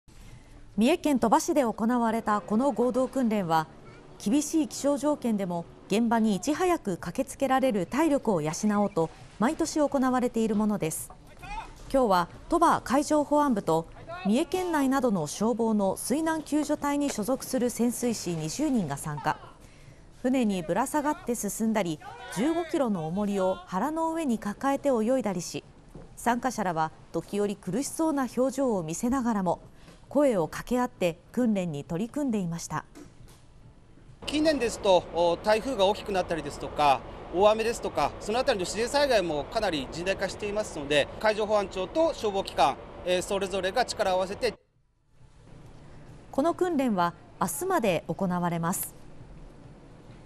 15キロの重りを抱えて泳ぐ…海上保安庁と消防の潜水士が合同訓練 甚大化する自然災害に備えて体力作り
水難救助にあたる、海上保安庁と消防の潜水士が合同訓練を行いました。

三重県鳥羽市で行われたこの合同訓練は、厳しい気象条件でも現場にいち早く駆けつけられる体力を養おうと毎年行われているものです。28日は鳥羽海上保安部と、三重県内などの消防の水難救助隊に所属する潜水士20人が参加。船にぶら下がって進んだり、15キロの重りを腹の上に抱えて泳いだりし、参加者らは時おり苦しそうな表情を見せながらも声を掛け合って、訓練に取り組んでいました。（鳥羽海上保安部巡視船いすず 山下浩一郎船長）
 「近年台風が大きくなったり大雨だったり、そのあたりの自然災害もかなり甚大化していますので、海上保安庁と消防機関それぞれが力を合わせて（いきたい）」
この訓練は29日まで行われます。